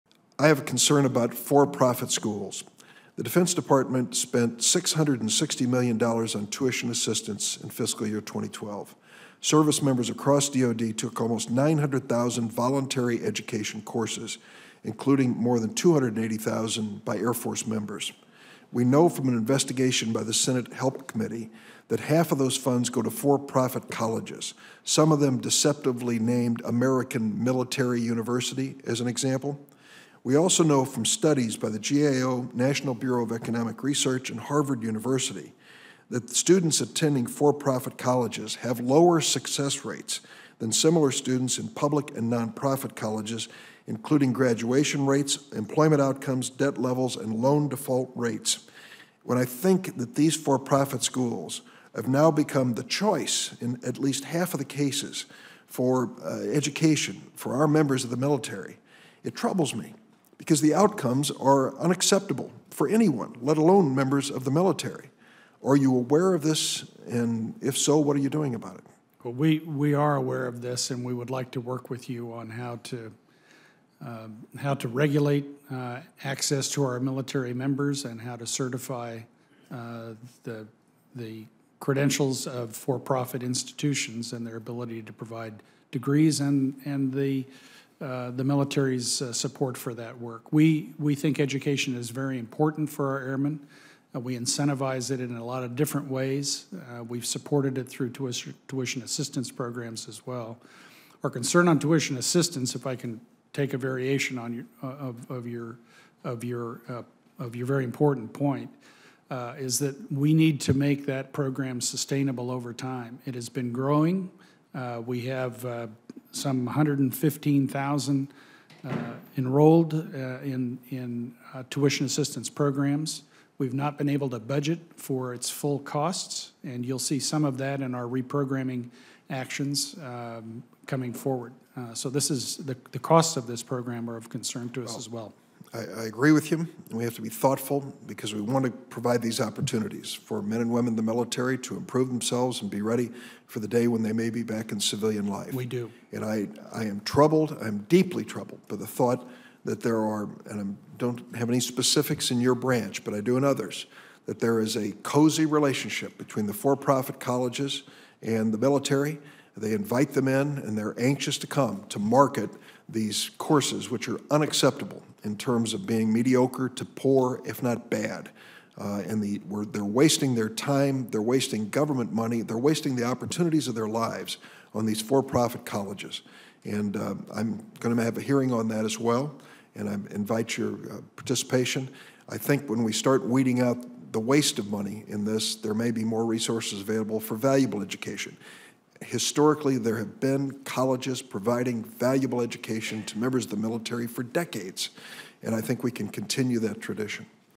Raising Concerns Regarding For-Profit Colleges and DOD Tuition Assistance
[WASHINGTON, D.C.] -- U.S. Senator Dick Durbin (D-IL), Chairman of the Senate's Defense Appropriations Subcommittee, spoke today about concerns regarding for-profit colleges targeting veterans servicemembers during a hearing on the Air Force's FY2014 budget request today.